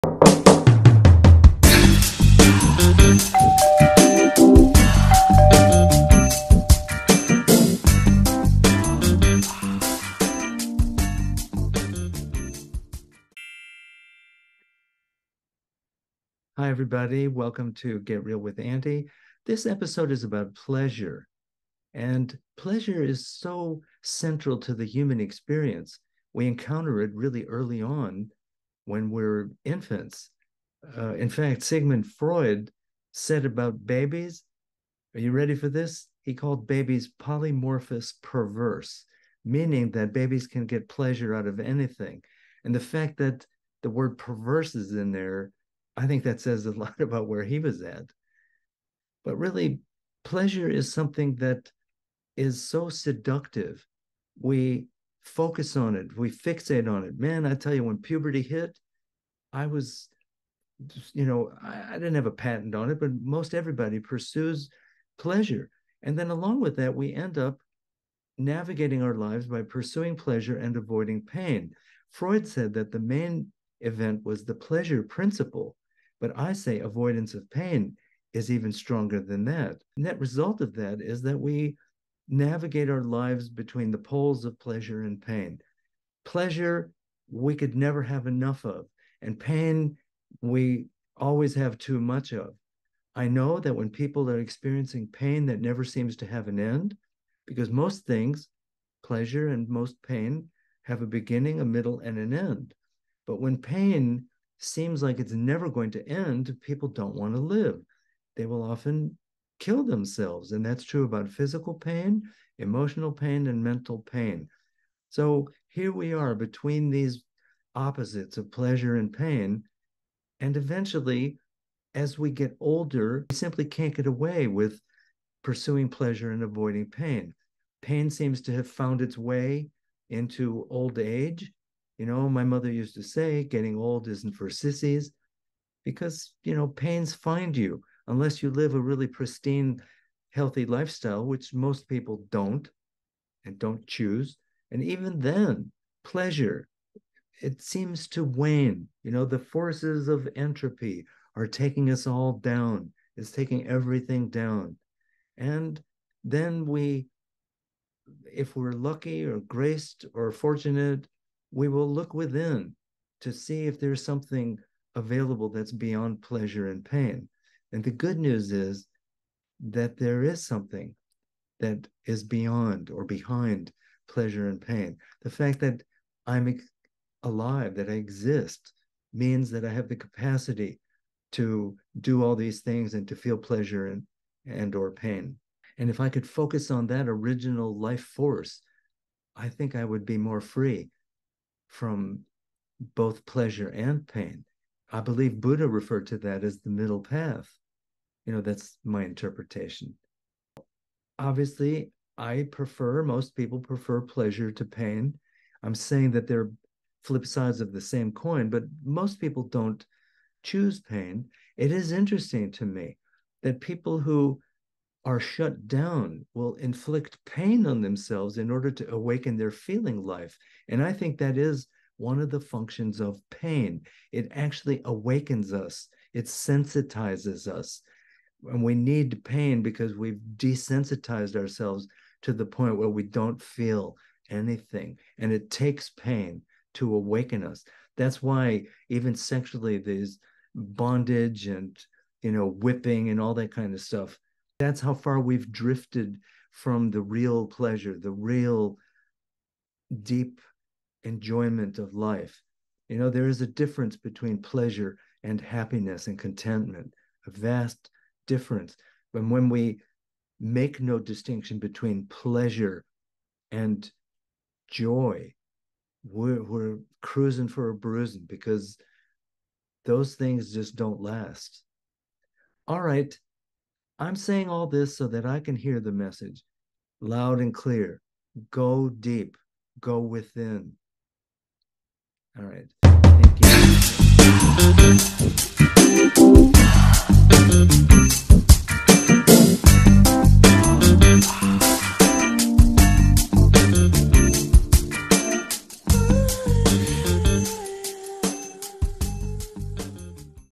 Get Real With Andy: Pleasure
I like pleasure. I don't like pain. I navigate my life pursuing one and avoiding the other. Are there any other options? And wait till you hear what Freud said about babies!